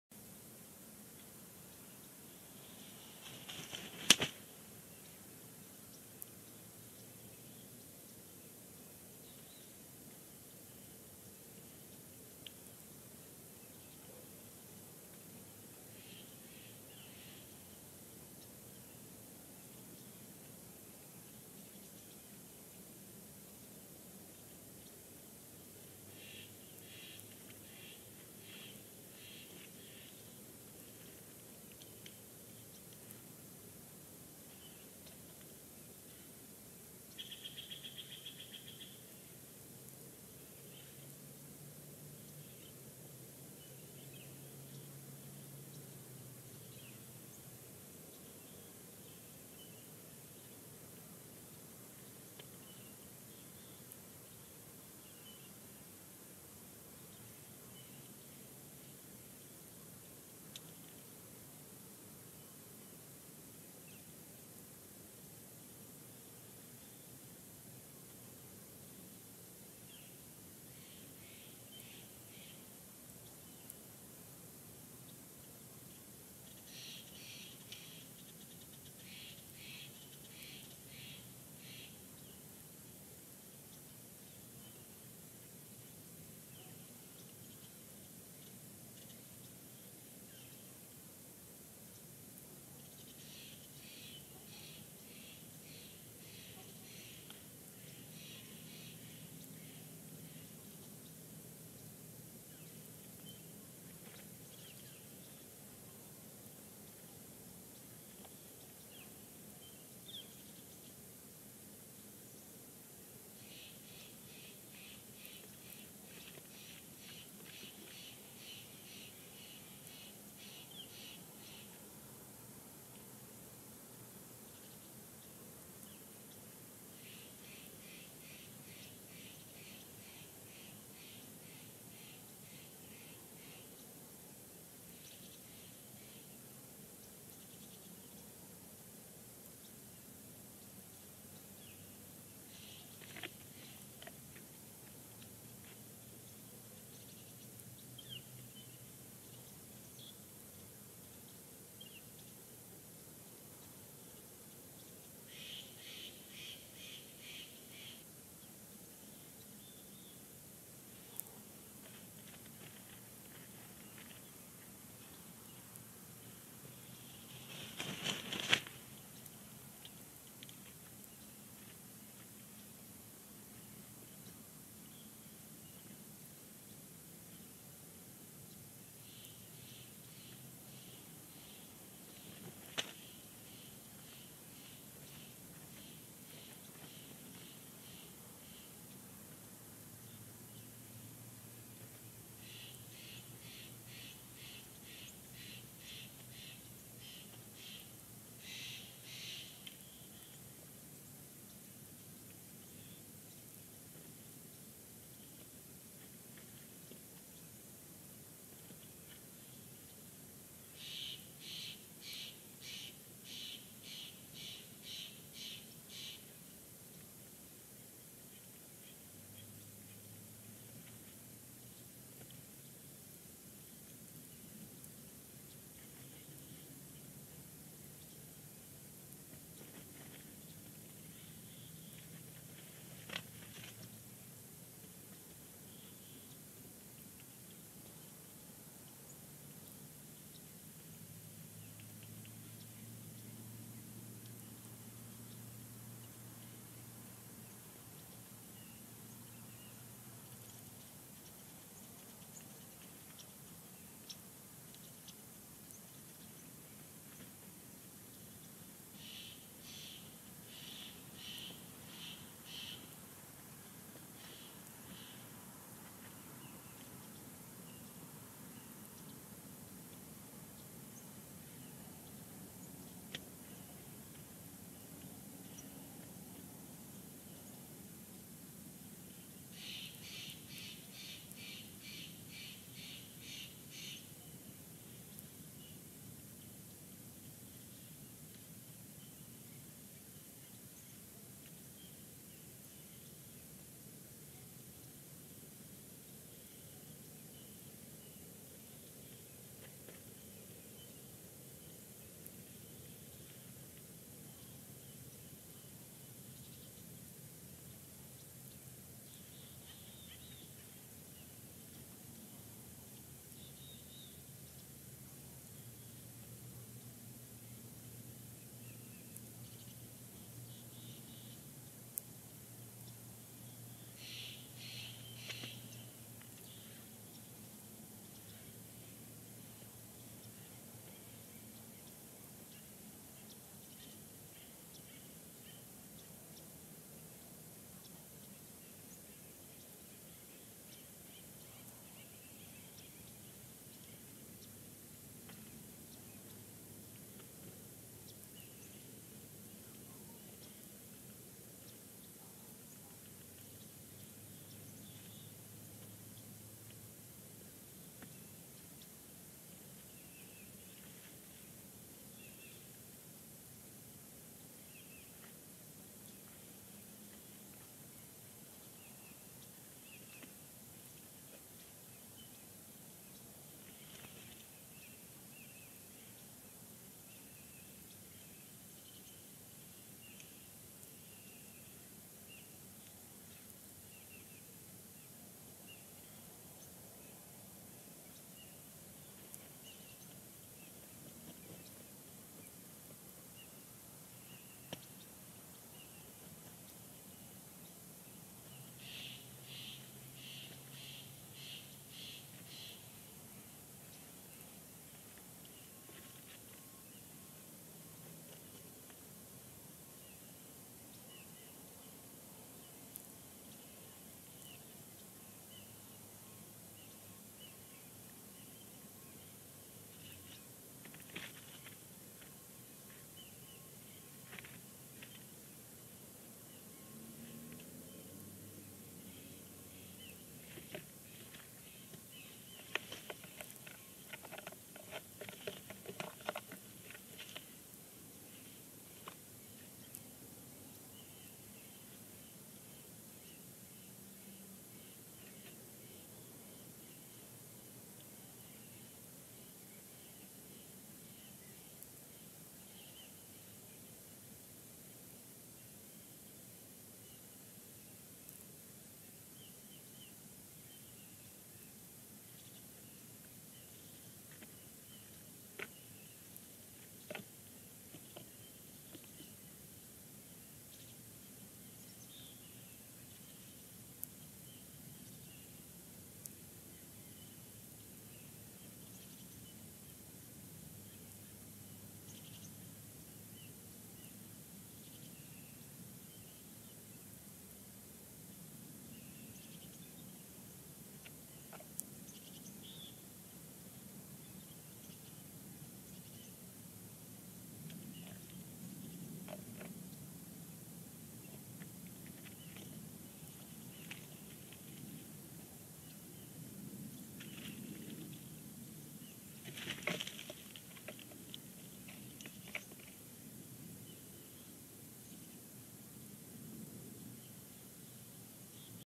11-22-19 Big Bear Lake eagles; a.m., Shadow in then Mrs. BB, part 1.
This is the first part of the morning work done by Shadow and Mrs. Big Bear.  First Shadow comes in, then Mrs. BB, and then they both work together.